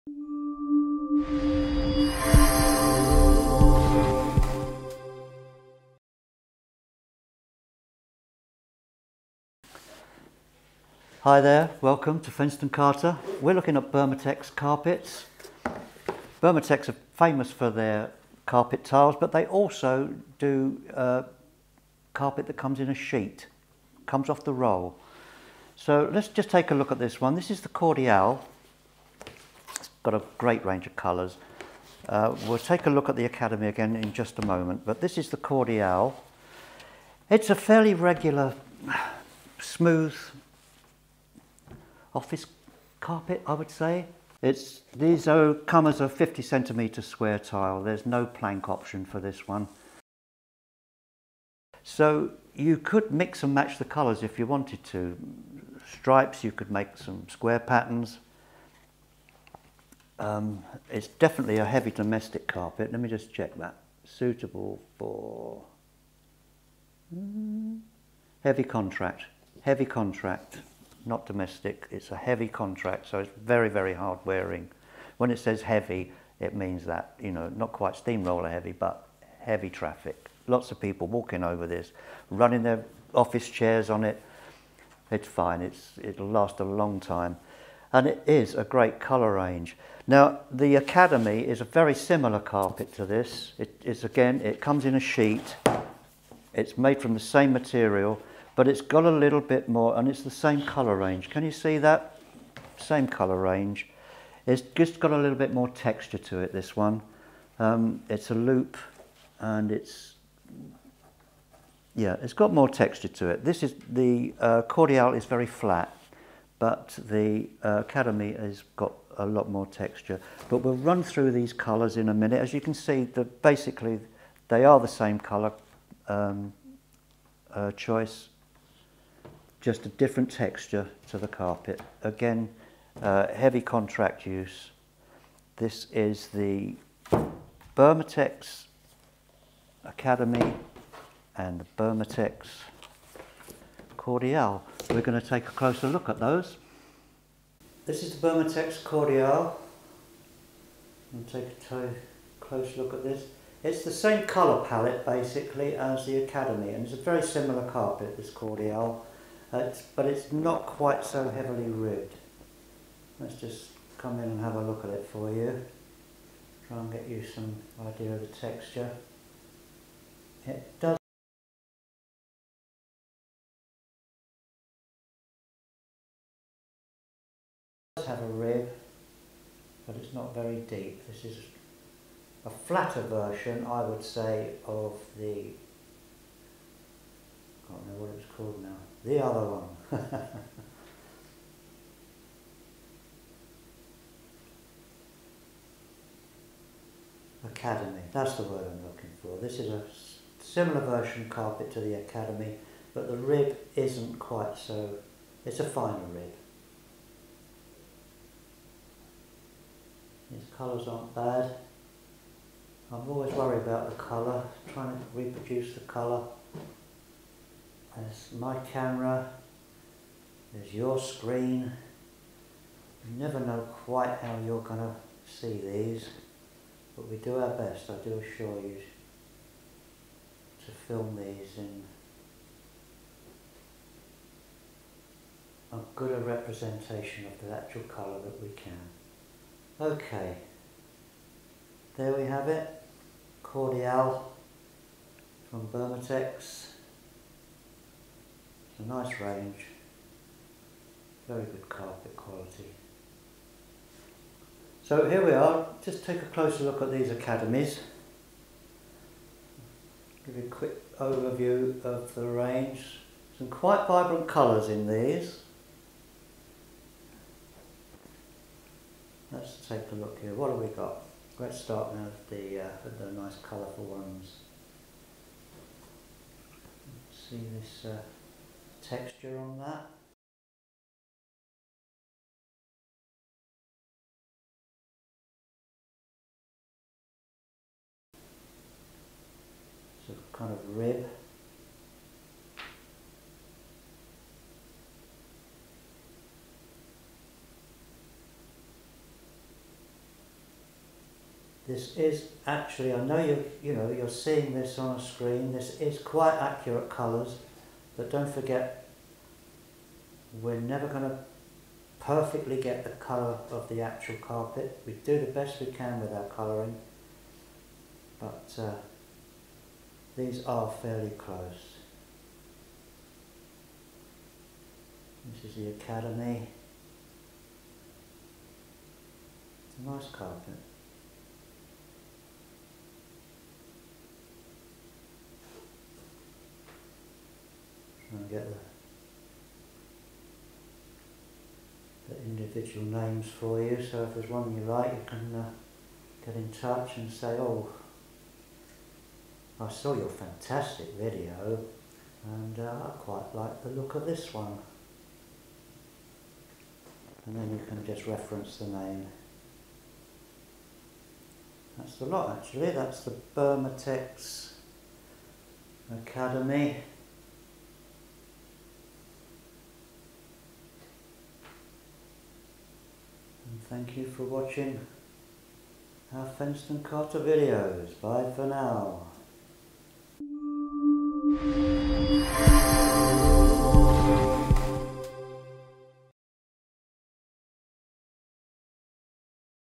Burmatex Cordiale and Academy office carpet tiles, a review
Burmatex Cordiale and Burmatex Cordiale

A hard-wearing, cheap and cheerful tile that does not compromise on quality. Extremely durable, fine-ribbed fibre bonded carpet tile with a nylon mix construction using solution dyed fibres. The durable finish is perfect for use in heavy footfall environments and it comes in a stunning and wide ranging colour bank. For the money, this carpet tile is very, very hard to beat.

Burmatex Academy.
Burmatex academy is a thick-ribbed fibre bonded carpet tile in a wide range of vibrant colours. Primarily designed for use across the education, contract and commercial sectors but it really can be used anywhere with a heavy contract commercial rating. A high-performance mix of solution dyed fibres this carpet will resist fading and depression imprints from chair castors.